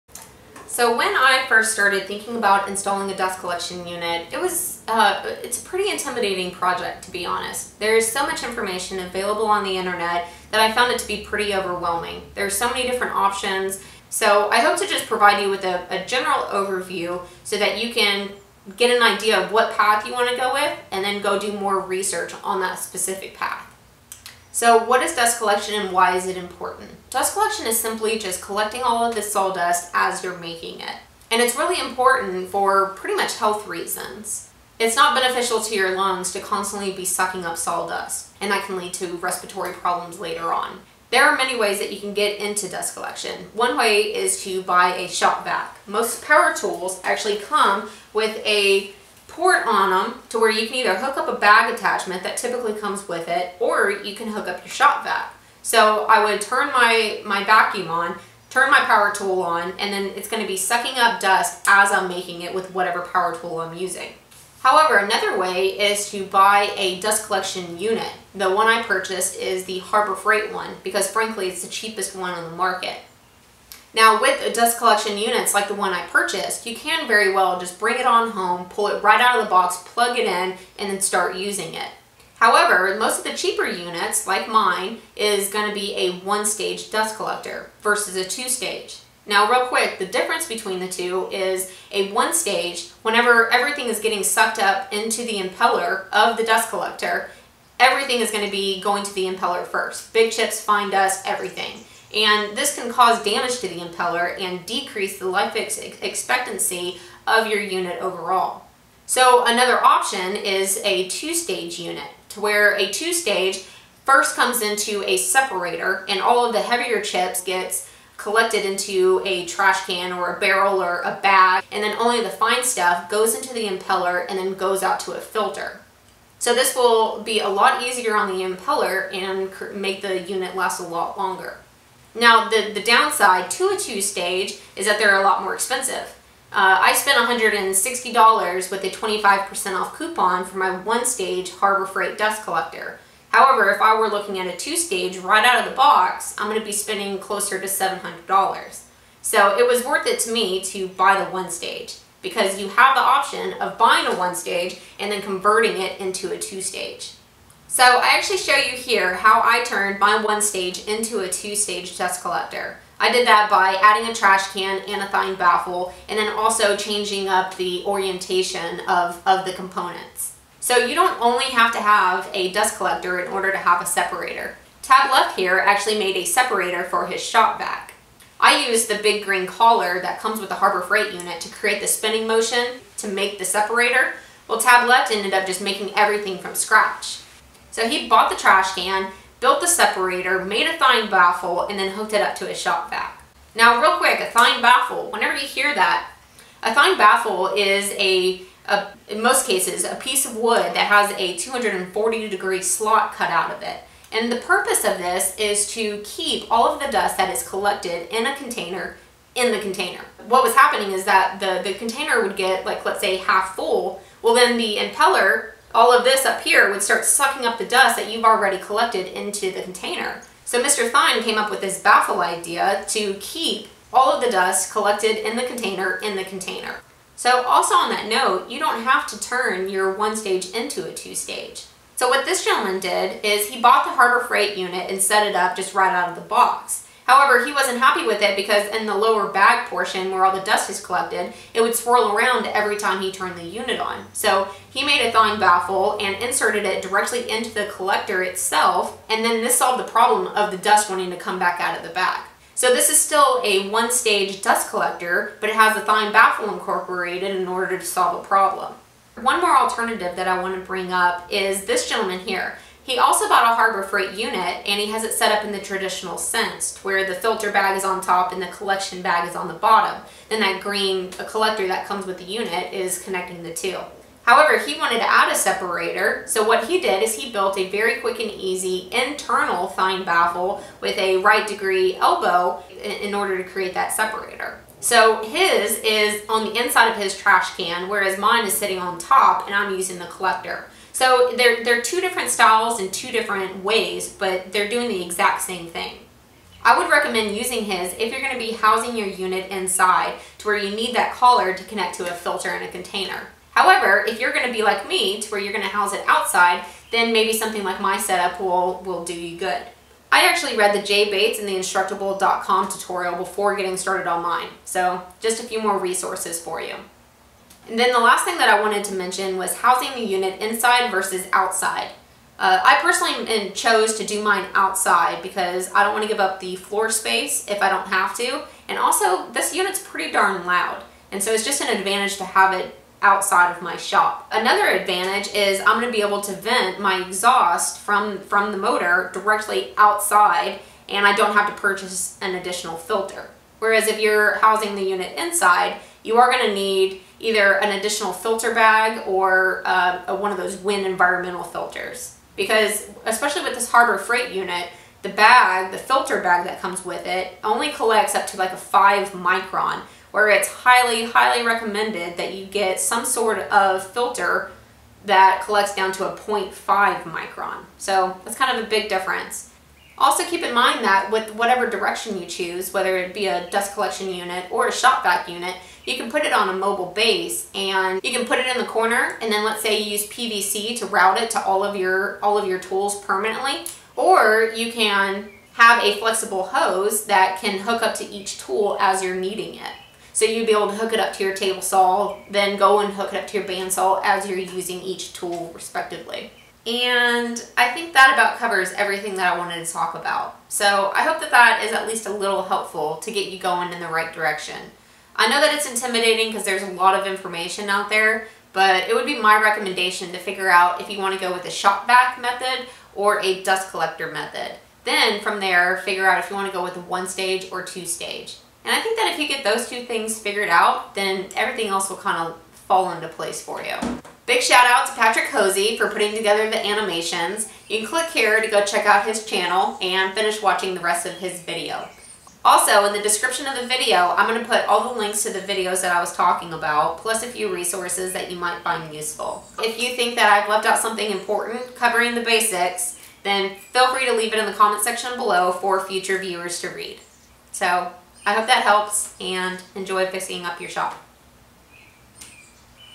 Basics of Dust Collection - Overview For Beginners
This video is me trying to explain a few different options and methods of dust collection for beginners.

Tableft: Makes a separator and Thien baffle from scratch then hooks it up to his shop vac

Vaughn: Keep his DC a one stage but adds a Thien baffle to the inside of the collector to control the dust collected in the bag

The Sergeant: Turns his one stage Harbor Freight DC into a two stage - makes an internal Thien baffle for his Harbor Freight unit

Jay Bates: Turns his one stage Harbor Freight DC into a two stage

Instructables: Guy turns his one stage Harbor Freight DC into a two stage

Phil Thien: Phil's website that go into a lot more detail on everything